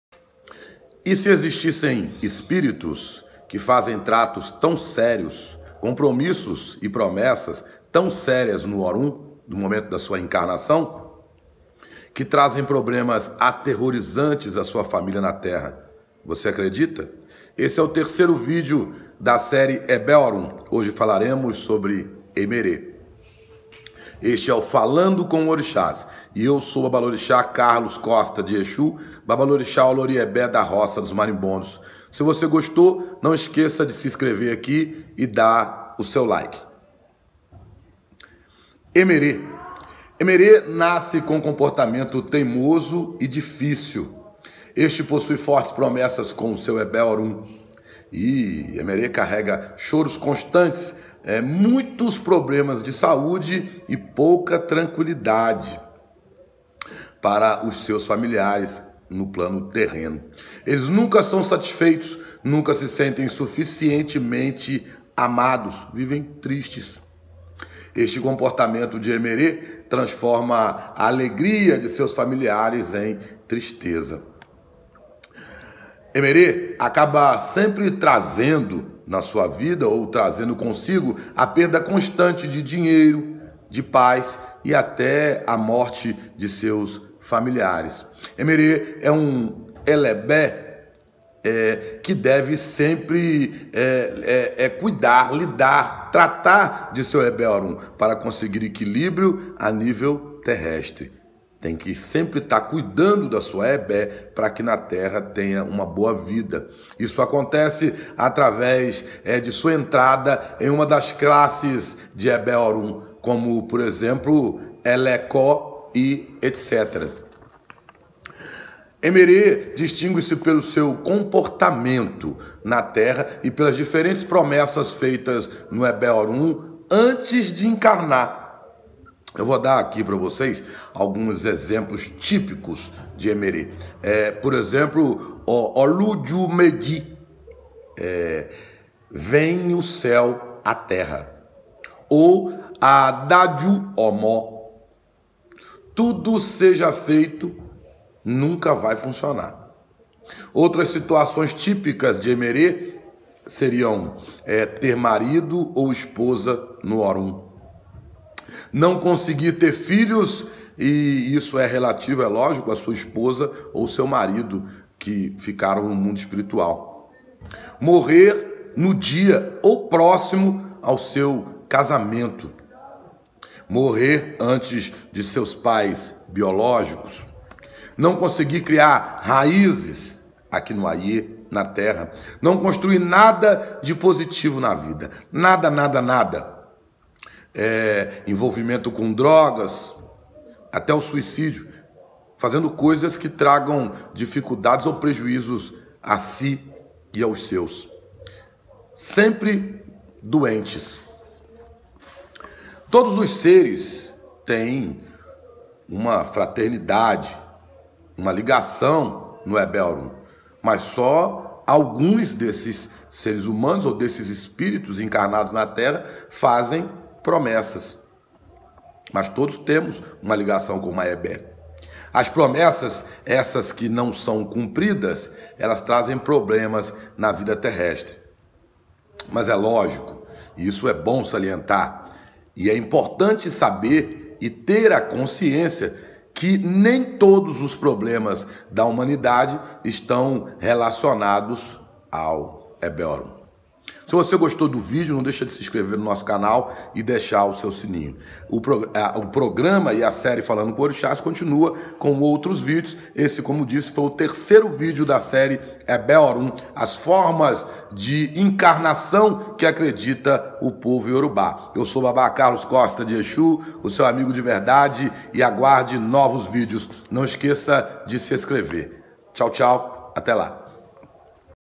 ÉMÉRÉ ÀBÍKÚ
Egbe Orun terceiro video da série formas de Reencarnação que acredita os Povos De Orisa